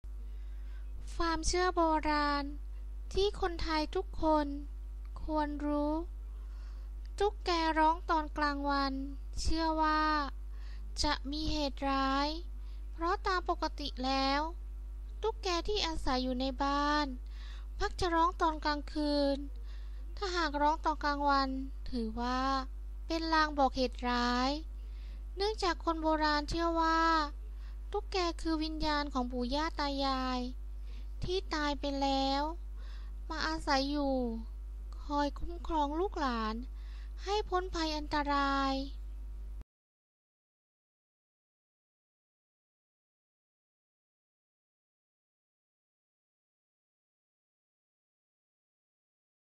ความเชื่อโบราณที่คนไทยทุกคน ควรรู้ ตุ๊กแกร้องกลวางวัน
ความเชื่อโบราณที่คนไทย ทุกคนควรรู้ ตุ๊กแกร้องกลางวัน ปกติตุ๊กแกจะร้องกลางคืน หากตุ๊กแกร้องกลางวันเป็นลางร้าย